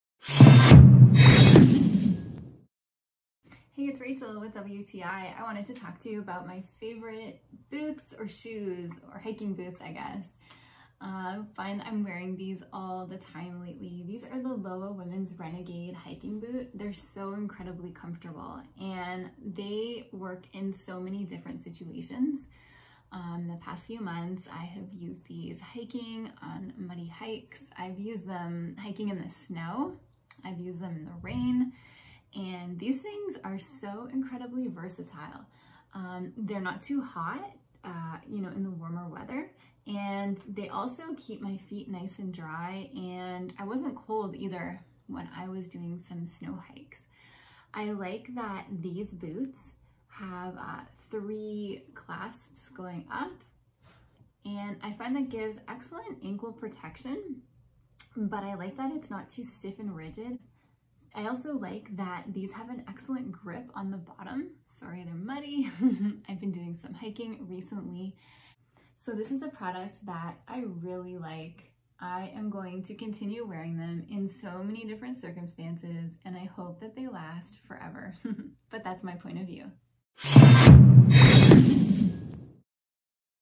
Our Point of View on Lowa Women's Bootie From Amazon
(Commissionable Links)

about this PRODUCT:
LOWA’s best selling style brings acclaimed trekking comfort fit to a multifunction construction using injected PU technology, MONOWRAP frame construction for stability and underfoot comfort. Full length stabilizer and VIBRAM Evo sole for sure-footed traction. Durably waterproof/breathable. Made on a women's specific last.